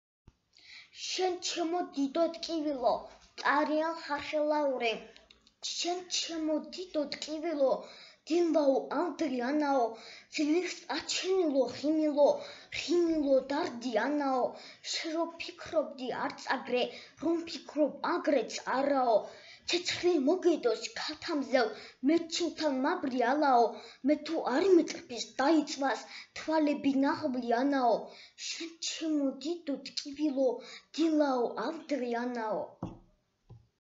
"შენ ჩემო დიდო... ტკივილო" - ტარიელ ხარხელაური
"შენ ჩემო დიდო... ტკივილო" - ტარიელ ხარხელაური. ლექსს კითხულობს ნიკო კარტოზია